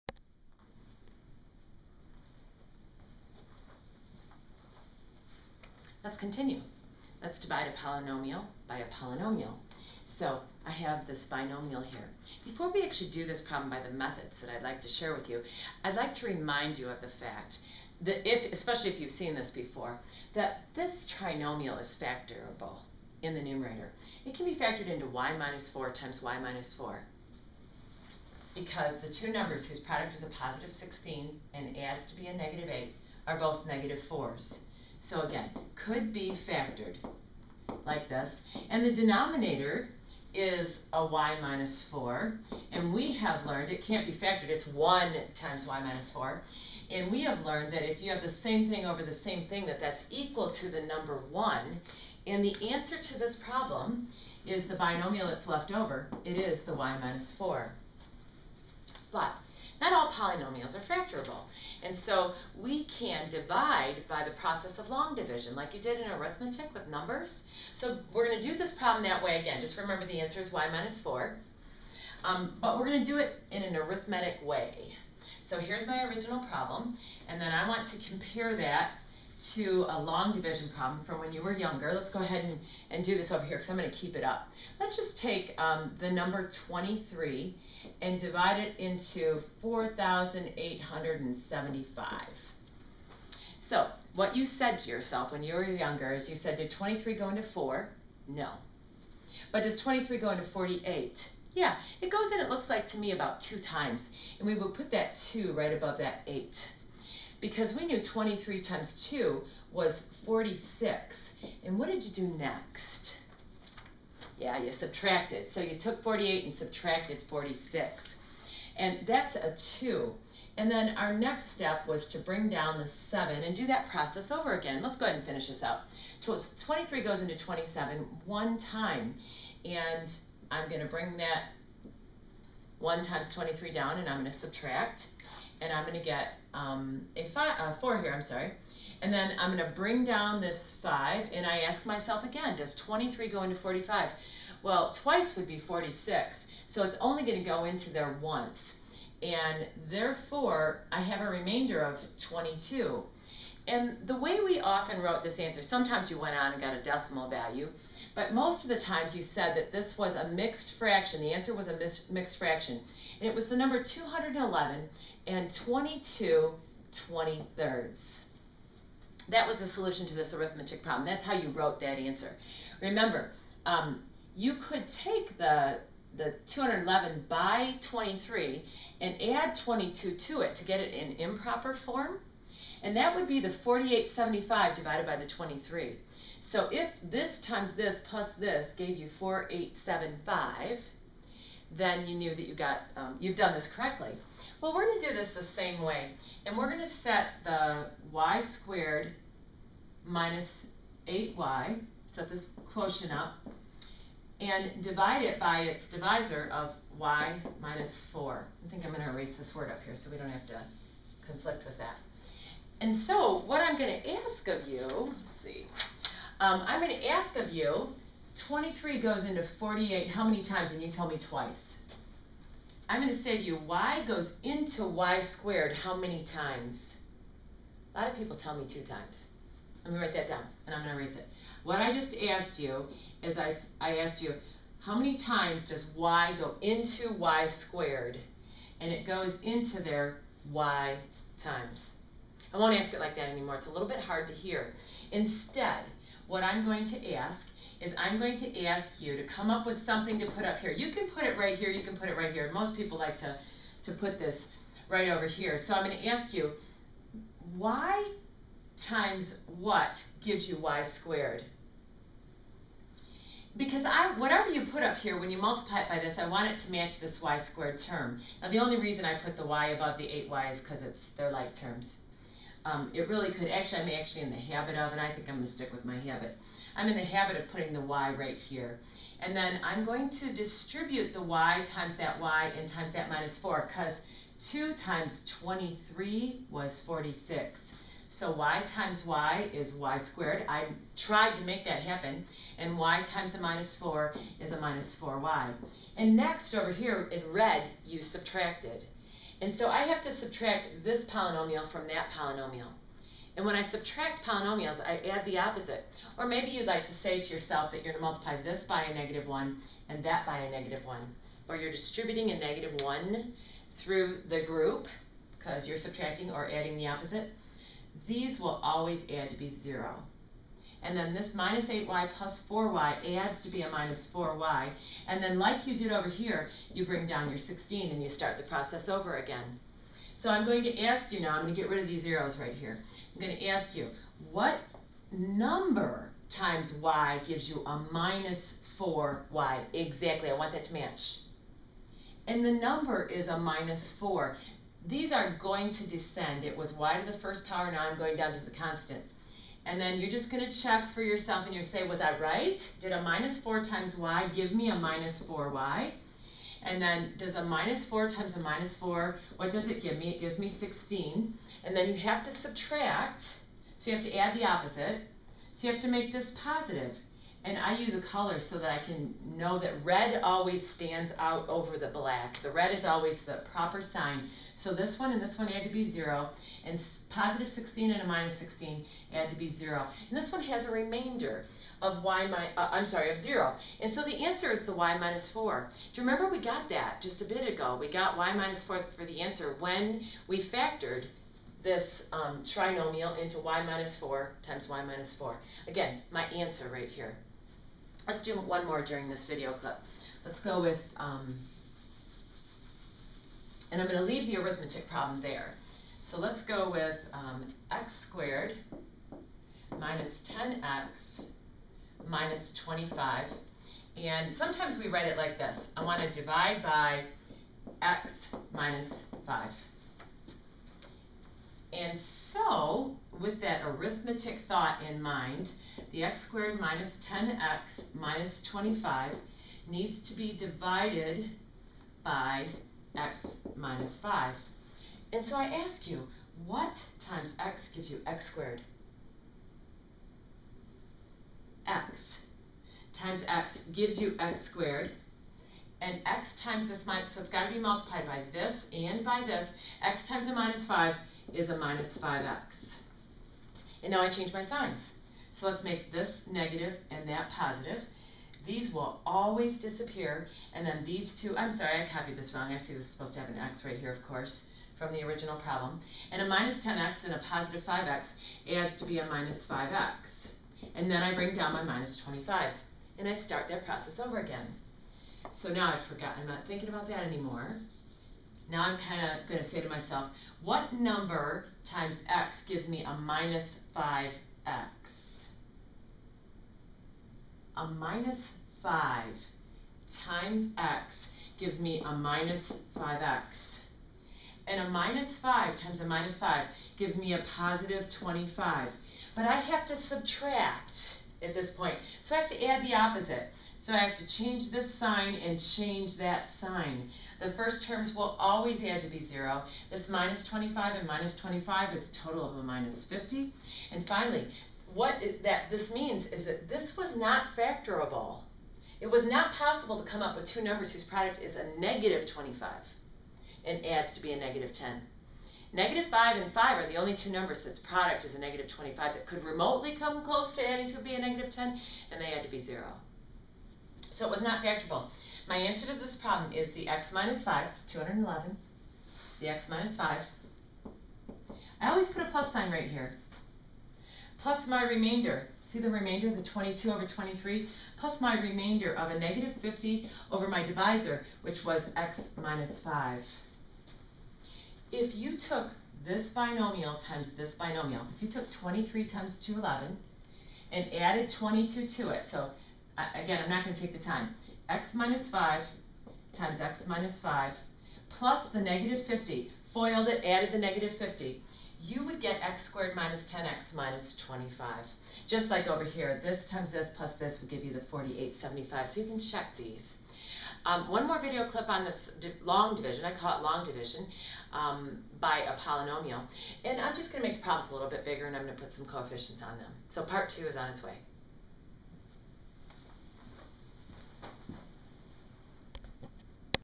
Divide Polynomial by Polynomial Part 1
A short video lecture on dividing polynomials by polynomials for Introductory & Intermediate Algebra students. Starfish award winning instructor Pat Kopf steps in front of the camera for a series of videos to support her Math 101 and 121 classes at Kellogg Community College.